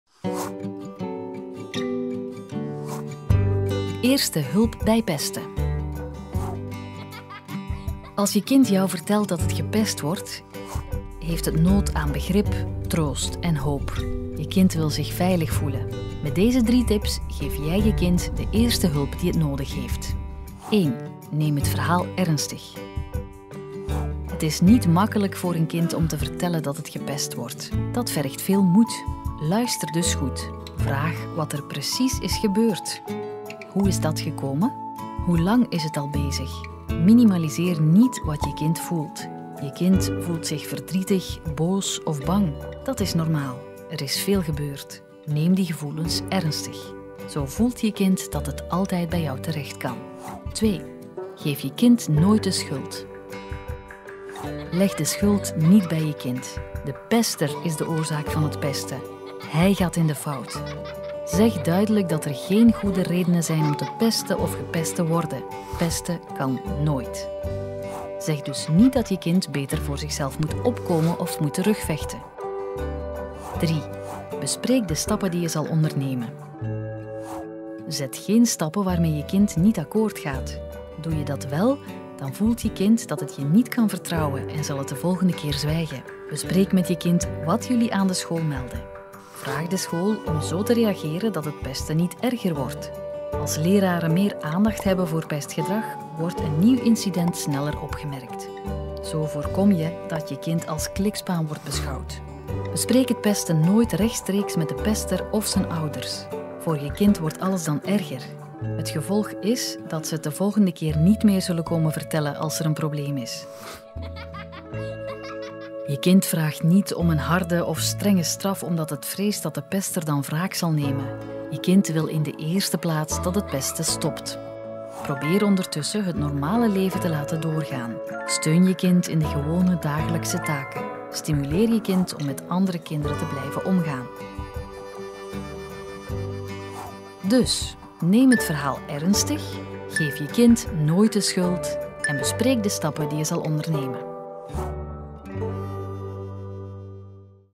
Als jouw kind gepest wordt: dit is wat jij kan doen // Ziezo! 4
Pesten op school samen de wereld uit helpen? Deze video vertelt wat je als ouder kan doen, als je kind vertelt dat het gepest wordt.

Elke week het beste van Klasse in je mailbox?